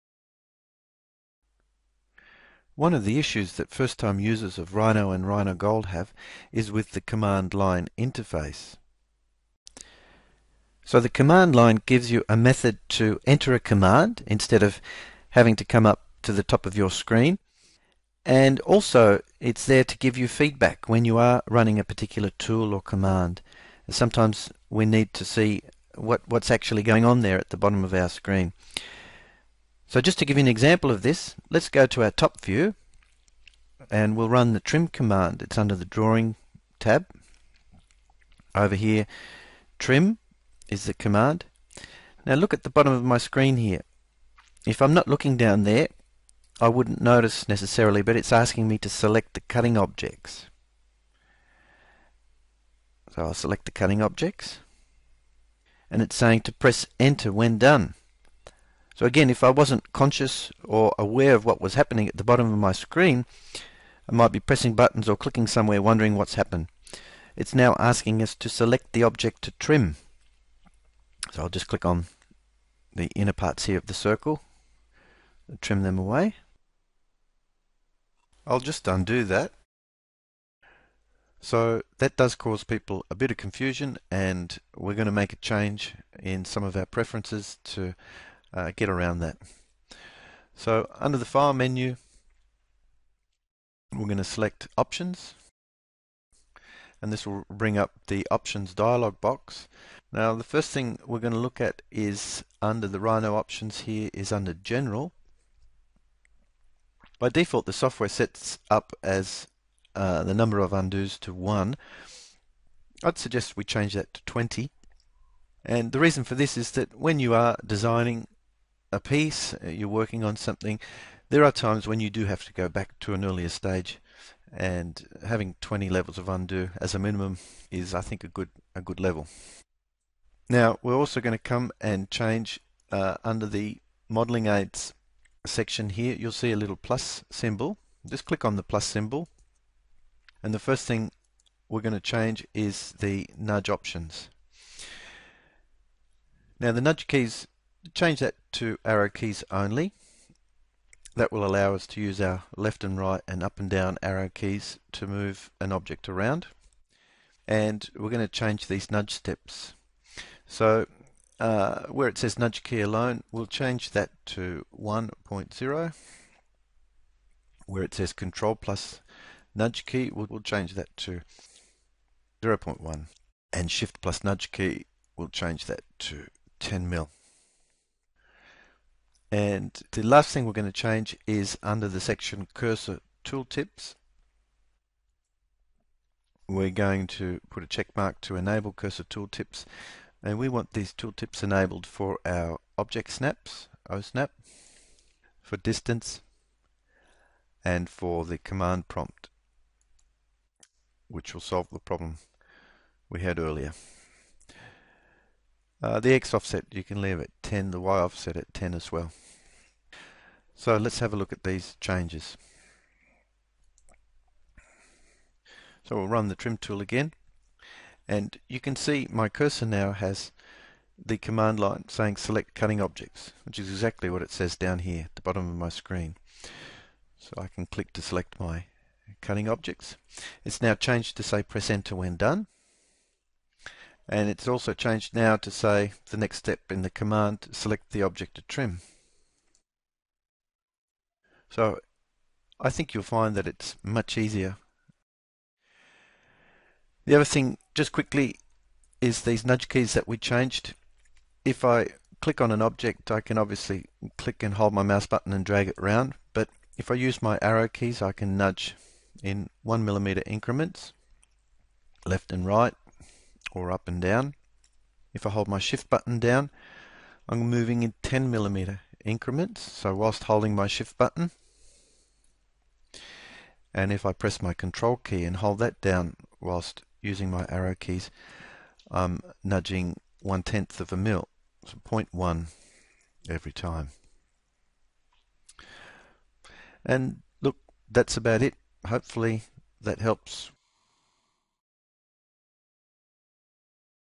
RhinoGold 4.0 Training - setting up preferences
In this video we show you how to setup some preferences in RhinoGold to assist you with modelling your designs in 3D. We cover tips to increase the max number of undo's and the setup of cursor tool tips and modelling aids.

For more videos and tutorials on RhinoGold 4.0, please visit us